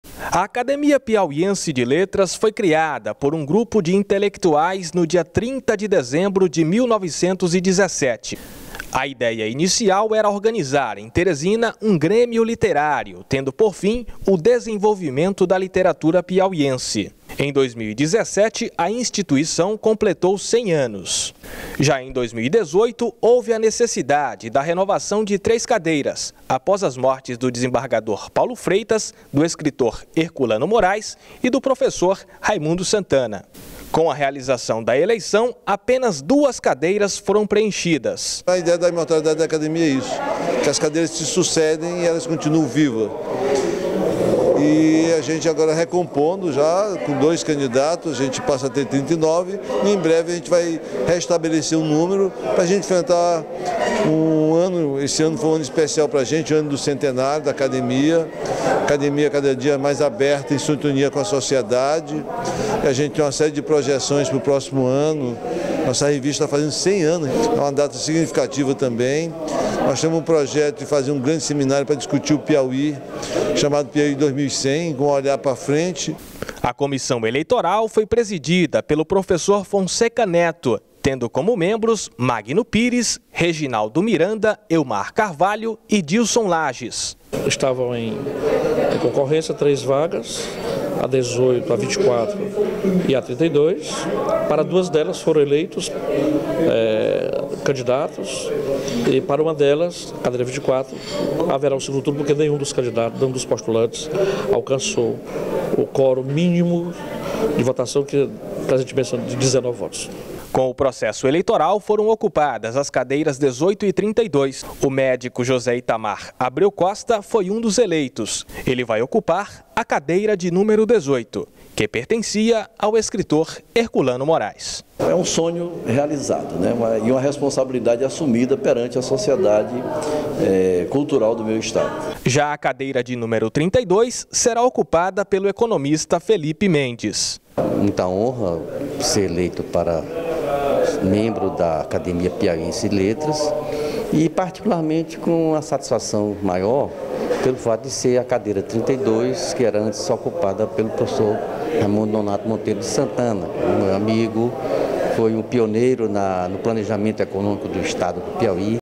O médico Itamar Abreu e o economista Felipe Mendes são os 2 novos imortais da APL
Membros da Academia Piauiense de Letras elegeram dois novos membros, o médico Itamar Abreu costa e o economista Felipe Mendes de Oliveira, os eleitos irão ocupar as cadeiras de números dezoito e trinta e dois, respectivamente